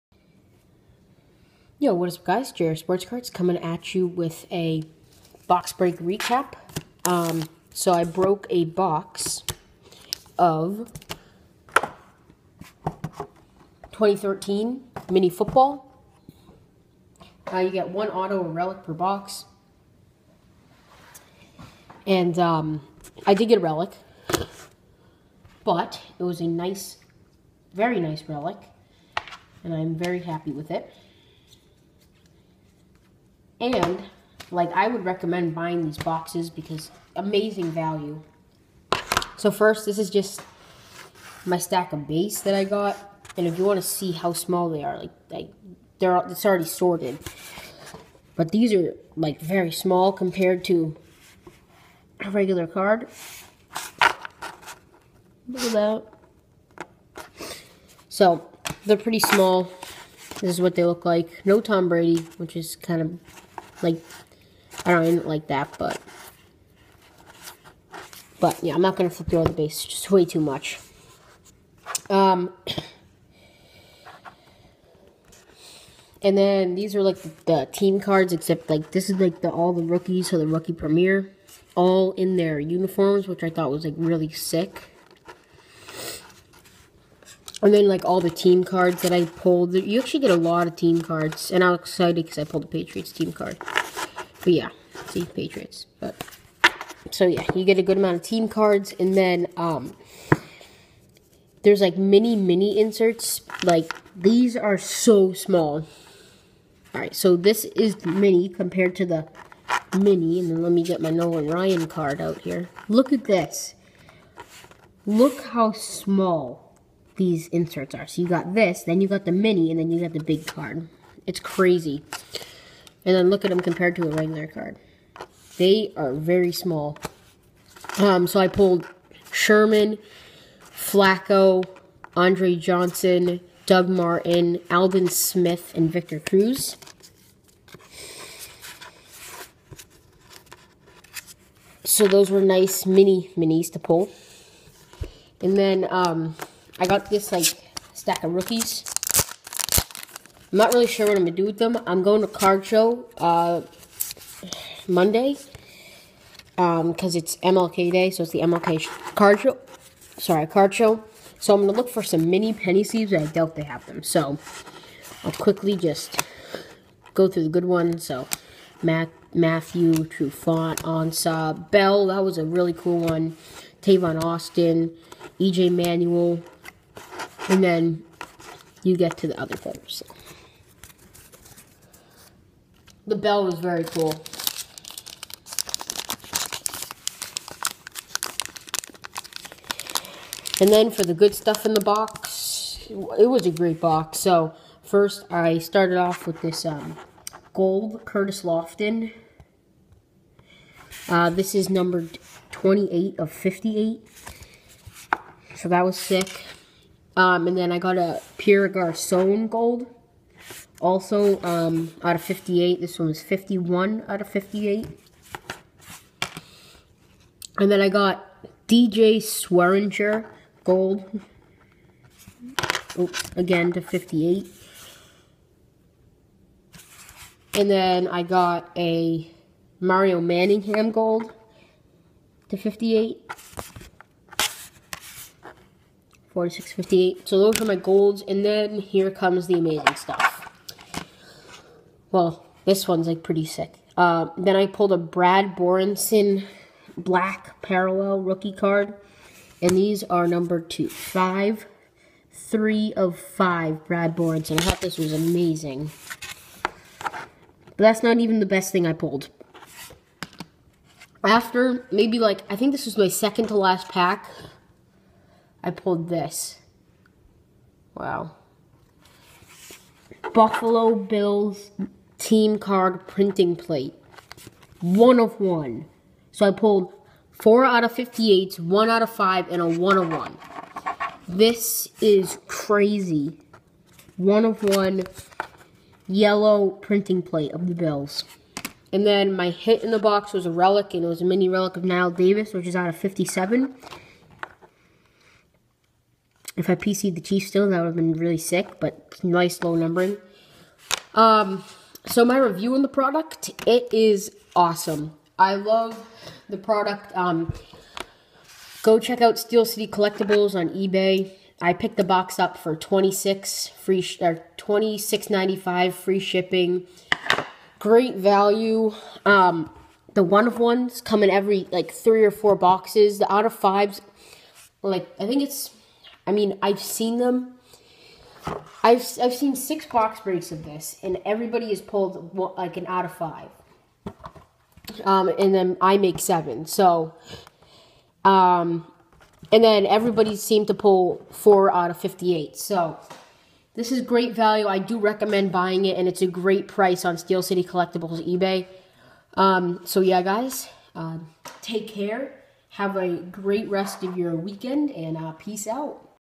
I PULLED A 1/1 AND A 3/5! | 2013 Topps Mini Football Box Break Recap
I post daily at 4:00pm est! Also, checkout my eBay- drsros1